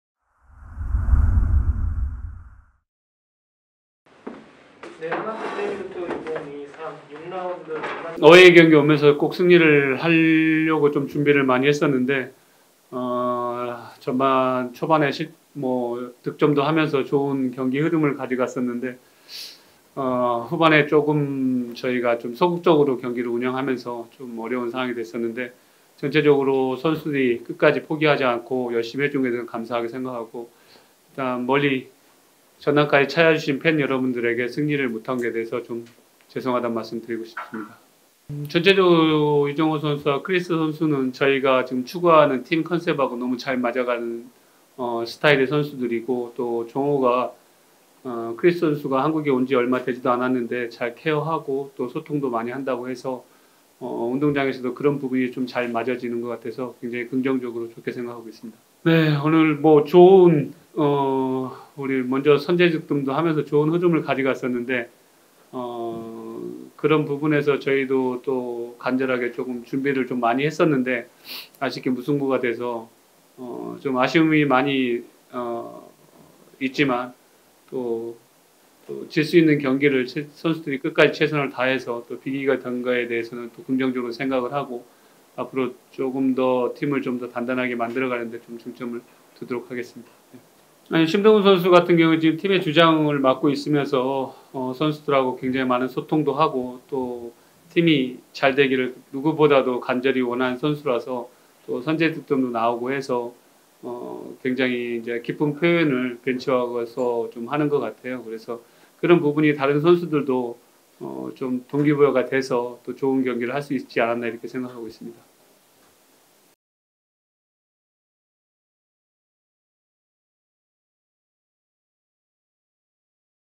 "전남까지 찾아와주신 팬 여러분께.." l 6R POST MATCH INTERVIEW 전남전 이기형 감독님 기자회견
4월 8일 6R 전남전 경기 후 성남FC 이기형 감독님의 기자회견 영상입니다.

✔까치군단의 모든 영상을 한자리에